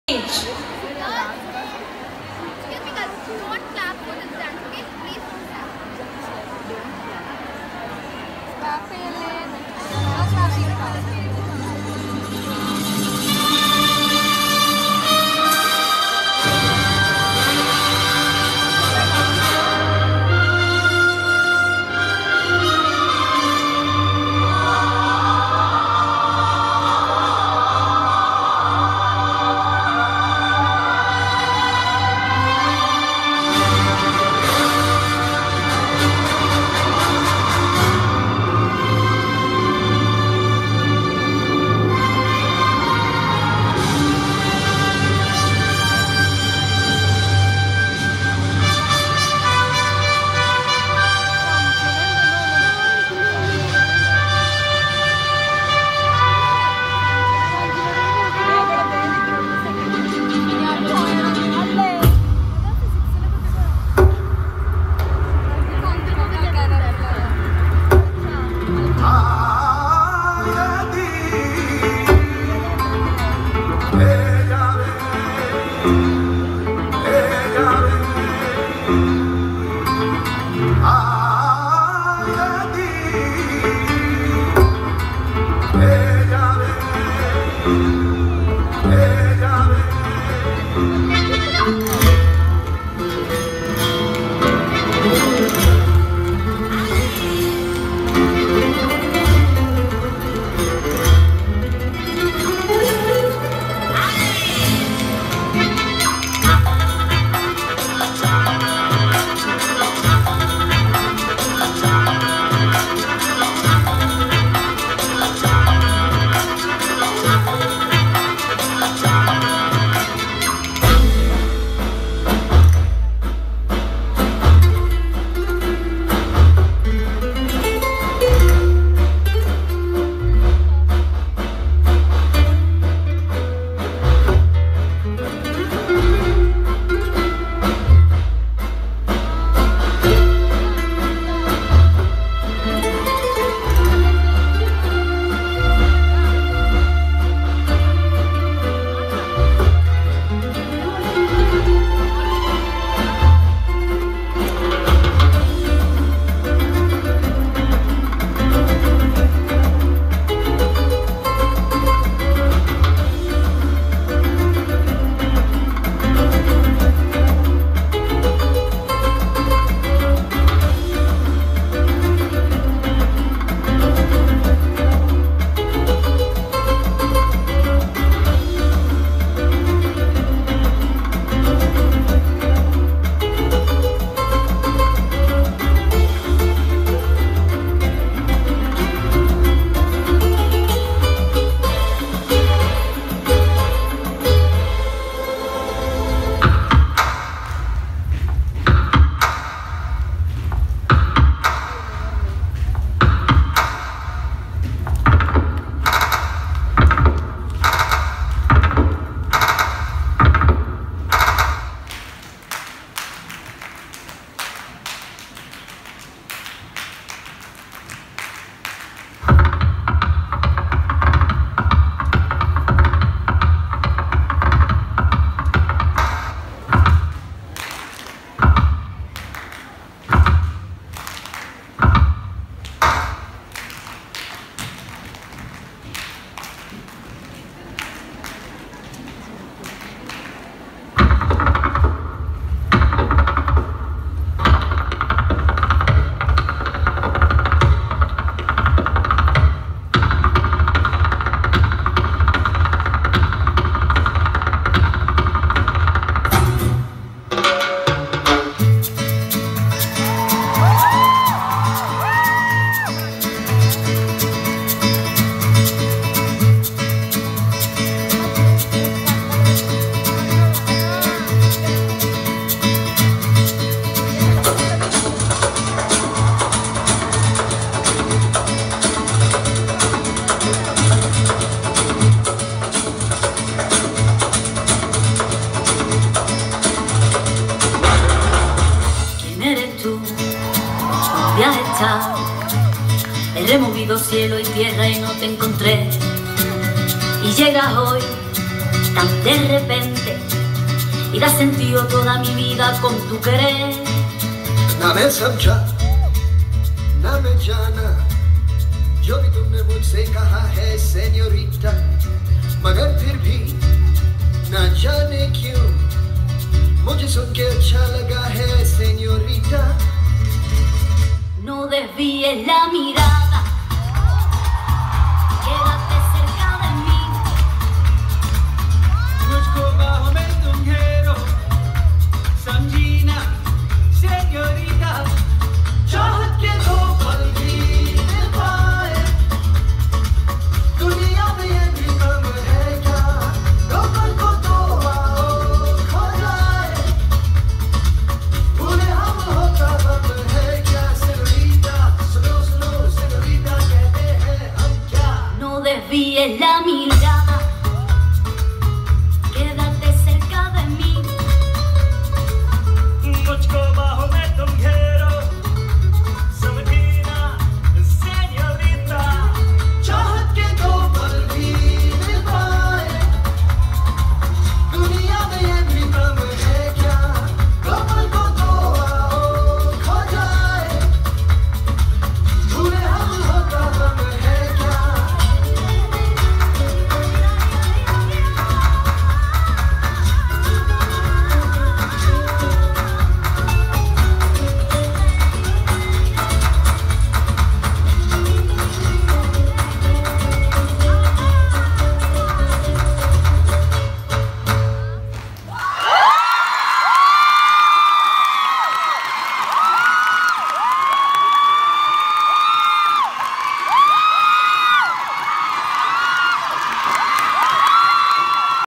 Flamenco dance (spanish dance ) by st teresas college 2016 - Rishika Paul
FLAMENCO DANCE (spanish) choreographed by sreejith master dancity .
featuring live clapping, live tapping
1st prize winners at st teresas college teresian week 2016